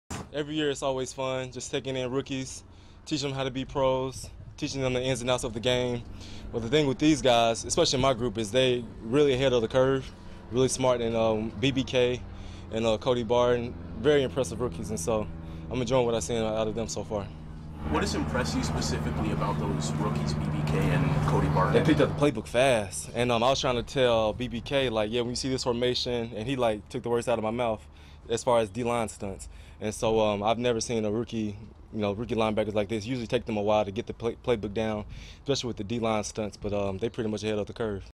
KJ Wright on Rookies 2019-05-29
KJ Wright says he is particularly pleased with rookie linebackers Cody Barton and Ben Burr-Kirven.  Wright spoke with reporters following the team's OTA's at the V-Mac in Renton Wednesday.